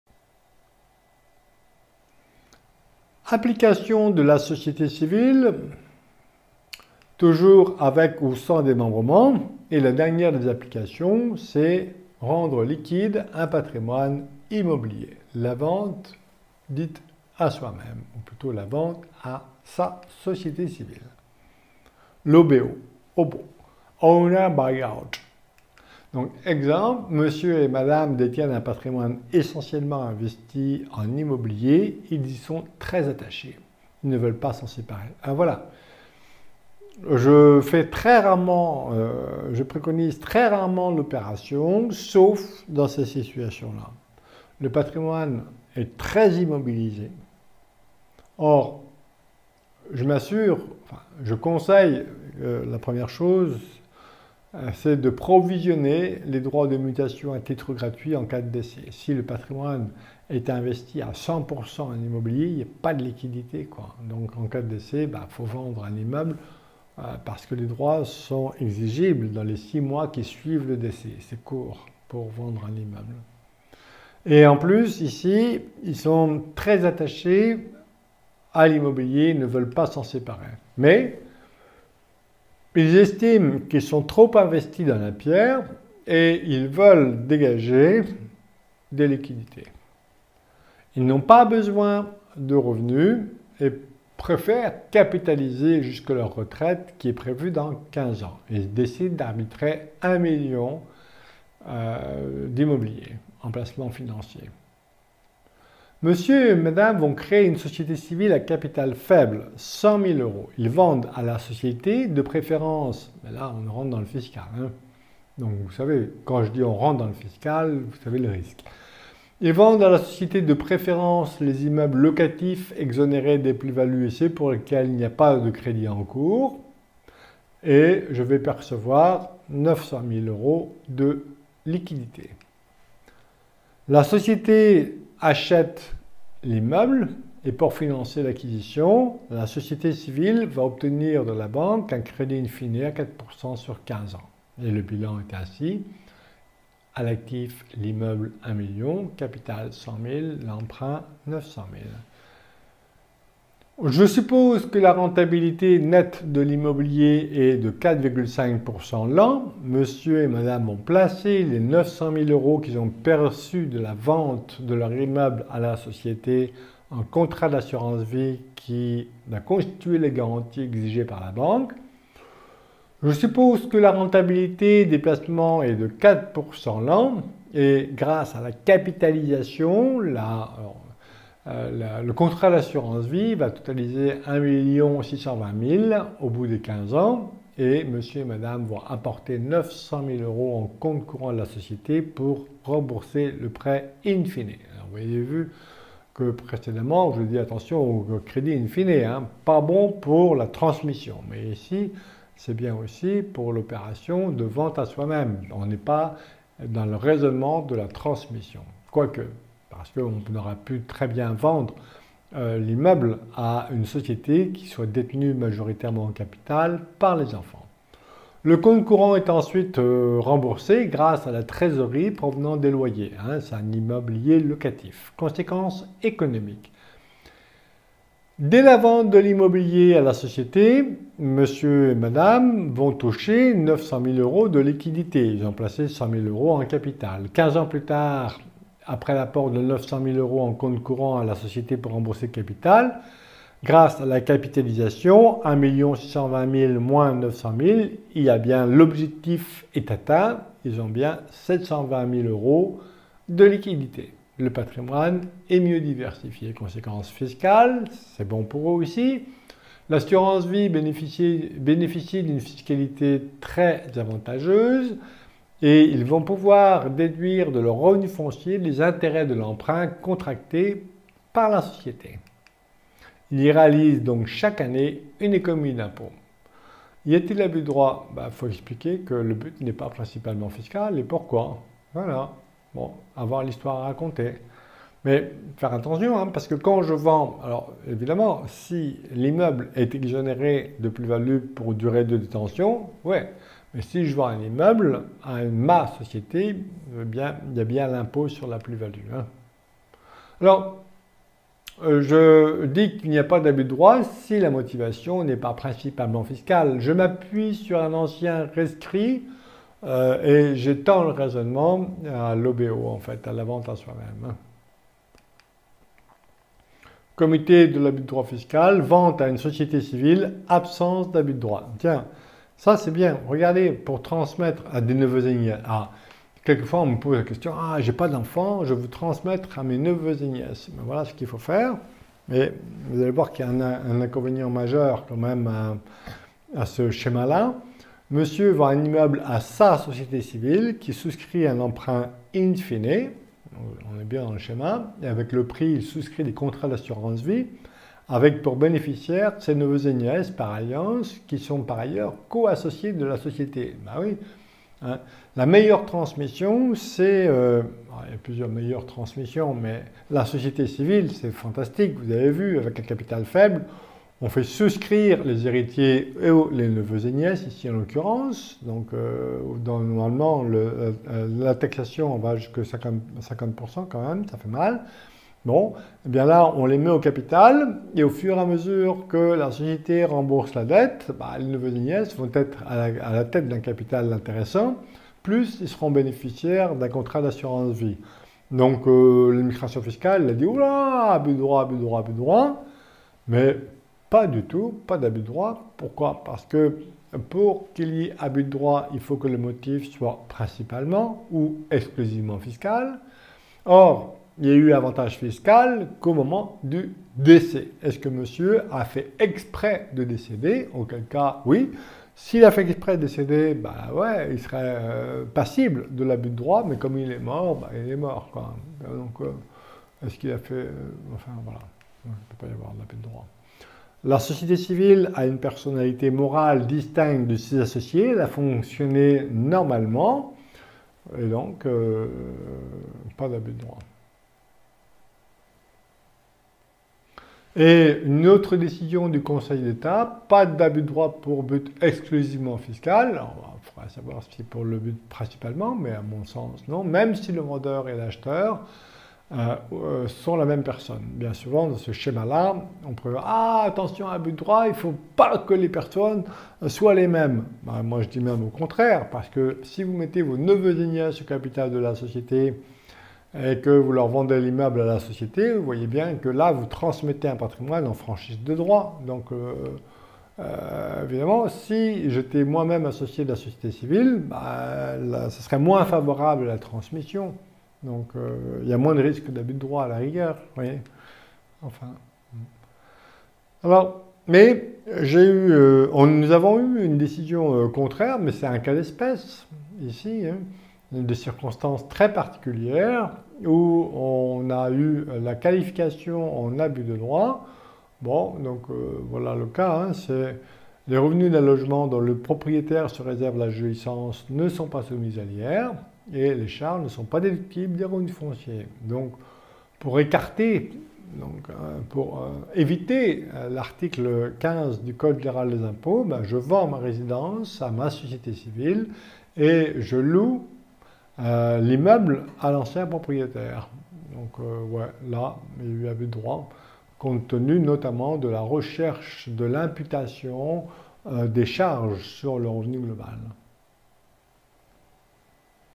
SCI vente à soi-même : vendre un immeuble à sa société civile, OBO Vidéo Youtube
Il est possible de vendre un immeuble que l'on détient déjà à sa société civile. Ce montage, appelé OBO, permet d'obtenir des liquidités dans son patrimoine.  Explications. La société civile emprunte afin d'acheter l'immeuble locatif. Le vendeur perçoit des liquidités, qu'il place en assurance-vie, le contrat servant de garantie pour la banque. Les intérêts de l'emprunt contracté par la société sont déductibles des revenus fonciers des associés. La vente à soi-même relève-t-elle de l'abus de droit fiscal ? Non si le but n'est pas principalement fiscal. D'ailleurs si on considère la fiscalité dans son ensemble, impôt sur le revenu, impôt sur les plus-values, droits d'enregistrement, droits de mutation à titre gratuit, IFI, la vente à soi-même est un schéma plus coûteux que d'autres. ▬▬▬▬▬▬  TOUS LES DOSSIERS

🎁Accéder librement à plus de 200 dossiers de Gestion de patrimoine

▬▬▬▬▬▬ INFORMATIONS DE CONTACT